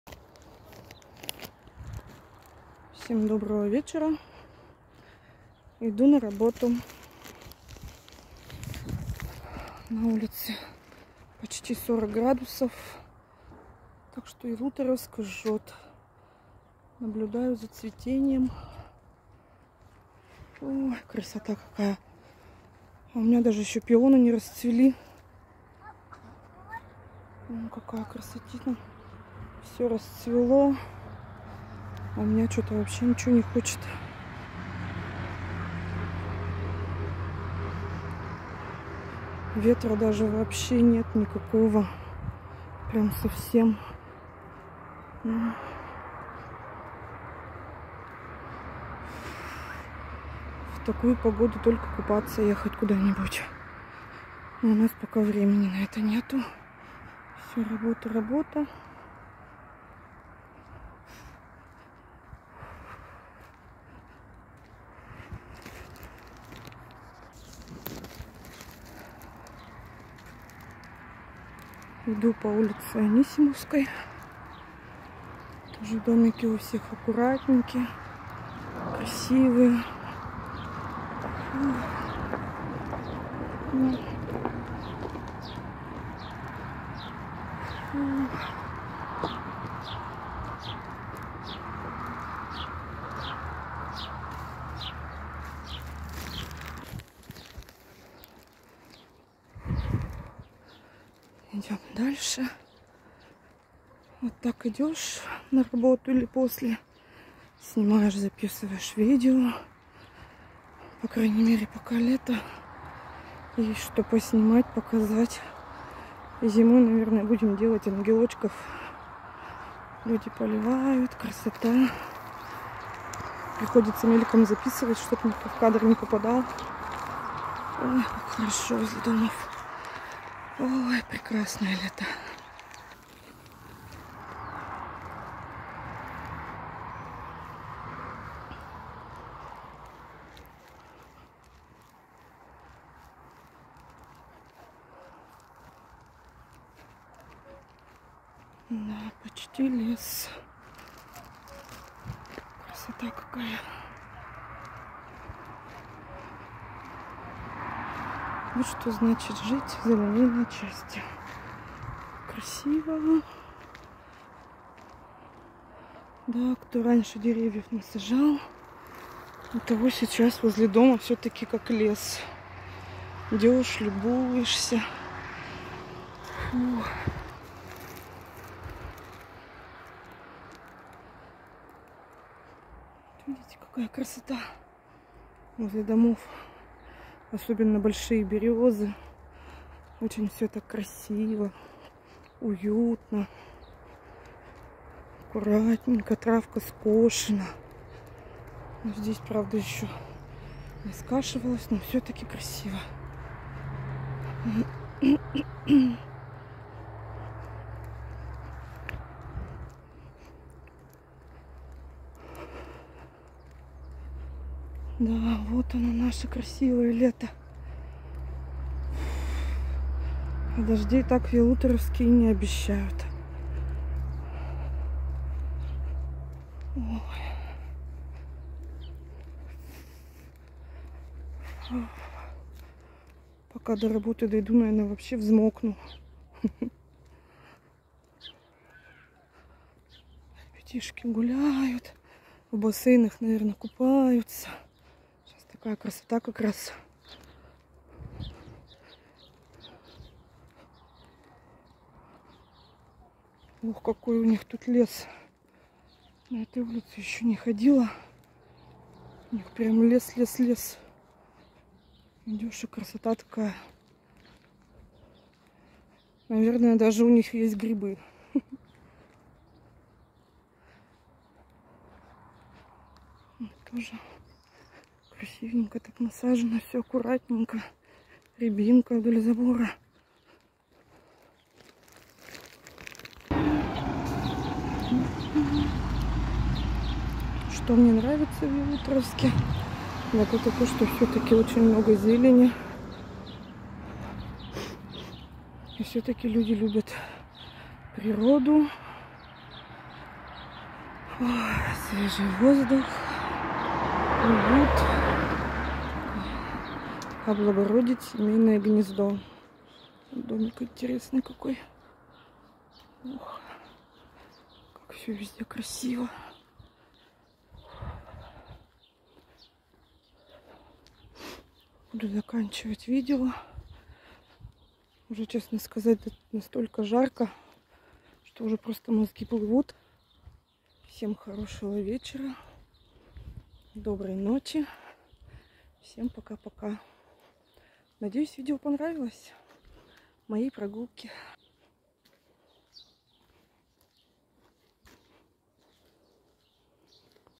Пешком на работу!
Прогулка до работы в Ялуторовске жара!ялуторовск